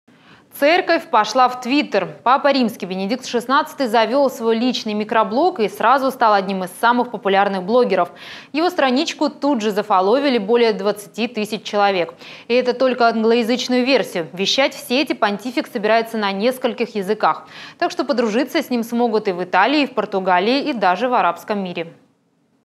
Зафоловить Папу
Церковь пошла в твиттер. Папа Римский Бенедикт XVI завел свой личный микроблог и сразу стал одним из самых популярных блоггеров. Его старничку тут же  зафоловили более 20 тысяч человек. И это только англоязычную версию. Вещать в сети понтифик собирается на нескольких языках, так что подружиться  ним смогут и в Италии, и в Португалии, и давже в арабском мире.